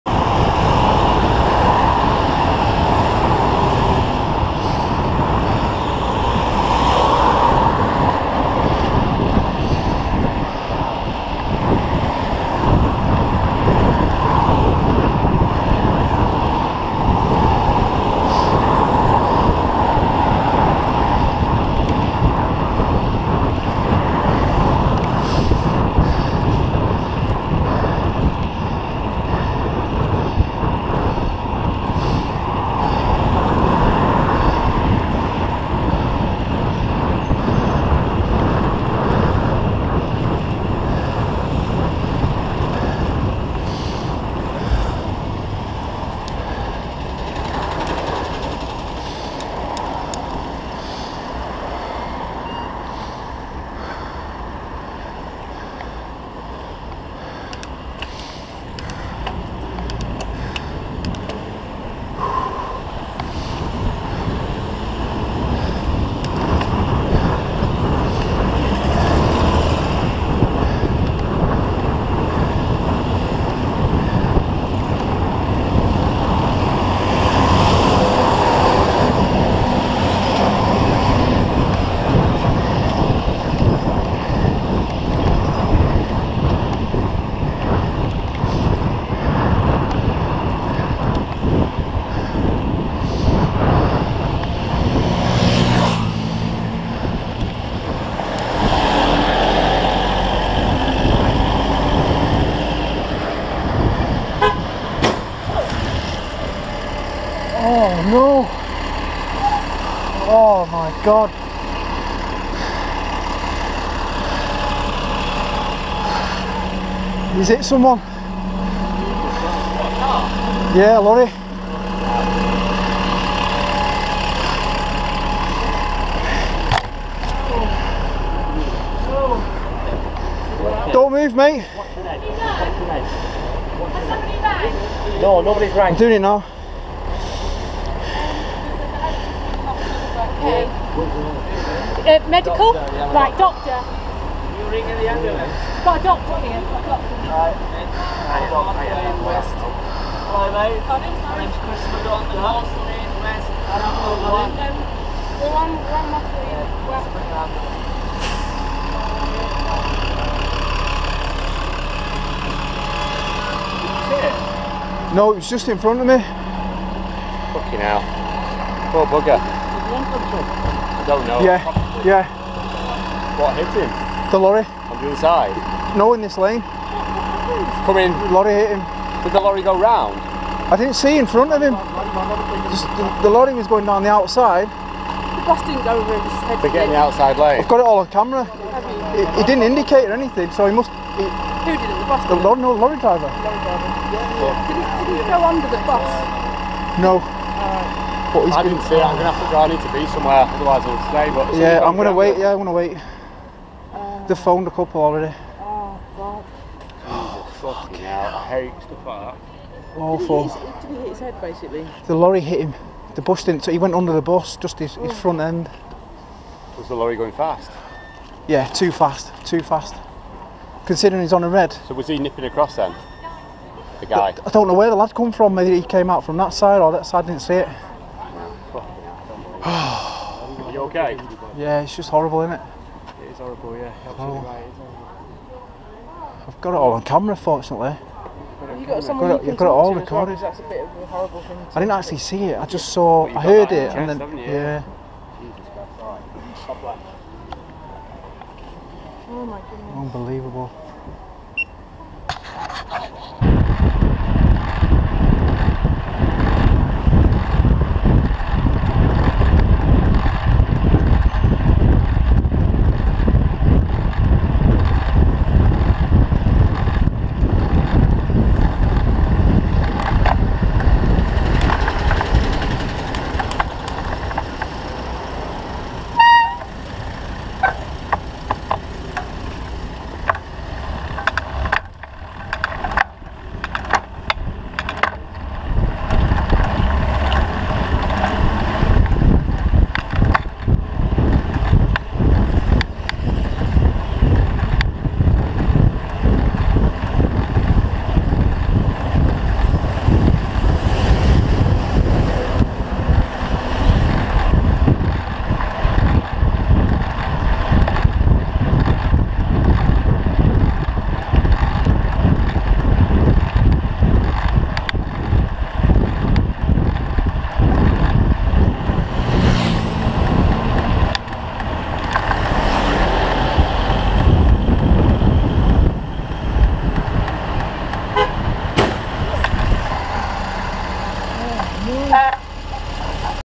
Man hit by lorry in Road Traffic Accident
All footage recorded using the GoPro White video camera .